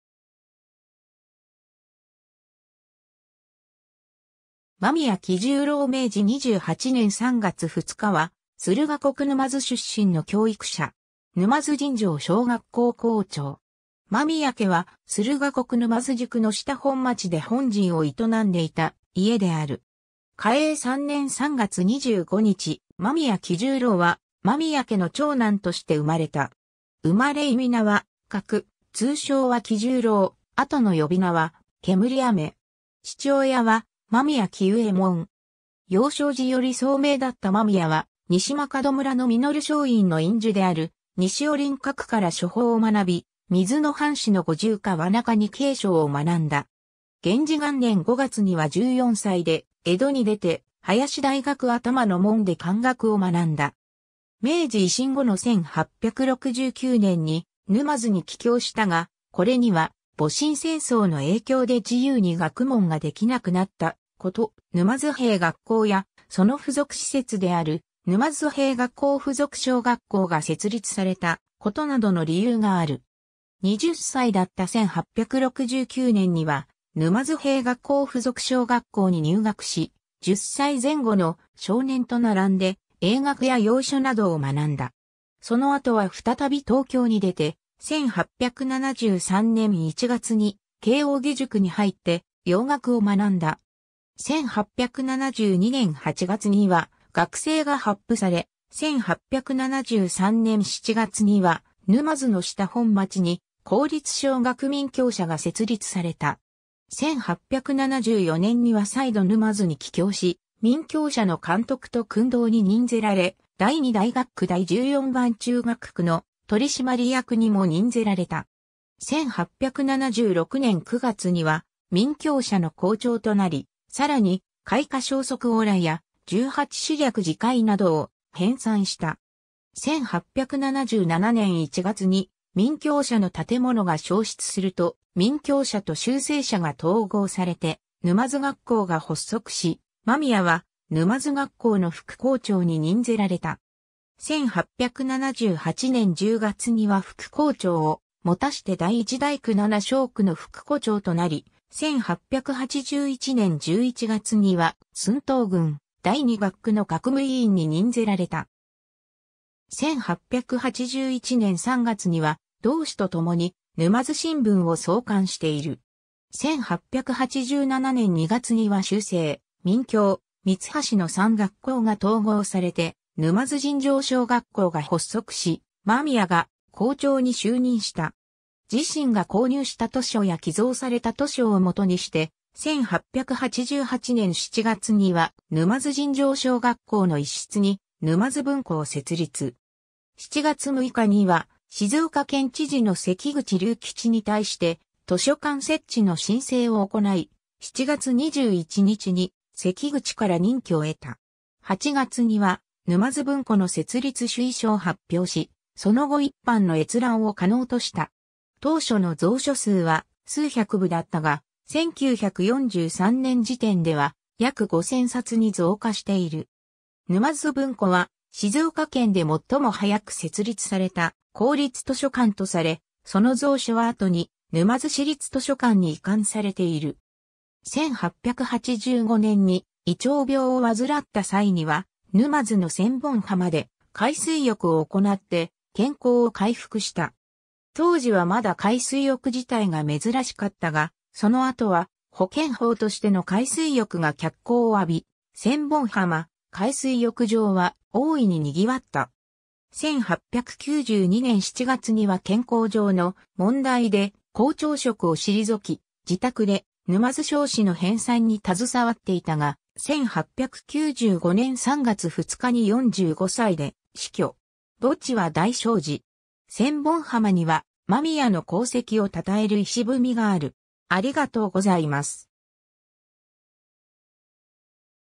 間宮喜十郎
間宮 喜十郎（まみや きじゅうろう、嘉永3年3月25日（1850年5月6日） - 明治28年（1895年）3月2日）は、駿河国沼津出身の教育者。
沼津尋常小学校（現・沼津市立第一小学校）校長。
 間宮家は駿河国沼津宿の下本町で本陣を営んでいた家である。
嘉永3年3月25日（1850年5月6日）、間宮喜十郎は間宮家の長男として生まれた。
生まれ諱は格、通称は喜十郎、後の呼び名は烟雨。
父親は間宮喜右衛門。
幼少時より聡明だった間宮は、西間門村の實相院の院主である西尾麟角から書法を学び、水野藩士の五十川中に経書を学んだ。
 元治元年（1864年）5月には14歳（数え年15）で江戸に出て、林大学頭の門で漢学を学んだ。
明治維新後の1869年（明治2年）に沼津に帰郷したが、これには戊辰戦争の影響で自由に学問ができなくなったこと、沼津兵学校やその附属施設である沼津兵学校付属小学校（現・沼津市立第一小学校）が設立されたことなどの理由がある。
20歳だった1869年（明治2年）には沼津兵学校付属小学校に入学し、10歳前後の少年と並んで英学や洋書などを学んだ。
その後は再び東京に出て、1873年（明治6年）1月に慶應義塾に入って洋学を学んだ。
 1872年（明治5年）8月には学制が発布され、1873年（明治6年）7月には沼津の下本町に公立小学明強舎が設立された。
1874年（明治7年）には再度沼津に帰郷し、明強舎の監督と訓導に任ぜられ、第二大学区第十四番中学区の取締役にも任ぜられた。
1876年（明治9年）9月には明強舎の校長となり、さらに『開化消息往来』や『十八史略字解』などを編纂した。
 1877年（明治10年）1月に明強舎の建物が焼失すると、明強舎と集成舎が統合されて沼津学校（現・沼津市立第一小学校）が発足し、間宮は沼津学校の副校長に任ぜられた。
1878年（明治11年）10月には副校長を持して第一大区七小区の副戸長となり、1881年（明治14年）11月には駿東郡第二学区の学務委員に任ぜられた。
1881年（明治14年）3月には同志とともに『沼津新聞』を創刊している。
 1887年（明治20年）2月には集成・明強・三橋の3学校が統合されて沼津尋常小学校（現・沼津市立第一小学校）が発足し、間宮が校長に就任した。
自身が購入した図書や寄贈された図書を基にして、1888年（明治21年）7月には沼津尋常小学校の一室に沼津文庫を設立。
7月6日には静岡県知事の関口隆吉に対して図書館設置の申請を行い、7月21日に関口から認許を得た。
8月には沼津文庫の設立趣意書を発表し、その後一般の閲覧を可能とした。
当初の蔵書数は数百部だったが、1943年（昭和18年）時点では約5000冊に増加している。
沼津文庫は静岡県で最も早く設立された公立図書館とされ、その蔵書は後に沼津市立図書館に移管されている。
 1885年（明治18年）に胃腸病を患った際には、沼津の千本浜で海水浴を行って健康を回復した。
当時はまだ海水浴自体が珍しかったが、その後は保健法としての海水浴が脚光を浴び、千本浜海水浴場は大いににぎわった。
 1892年（明治25年）7月には健康上の問題で校長職を退き、自宅で『沼津小誌』の編纂に携わっていたが、1895年（明治28年）3月2日に45歳（享年46）で死去。
墓地は大聖寺。
千本浜には間宮の功績を称える碑がある。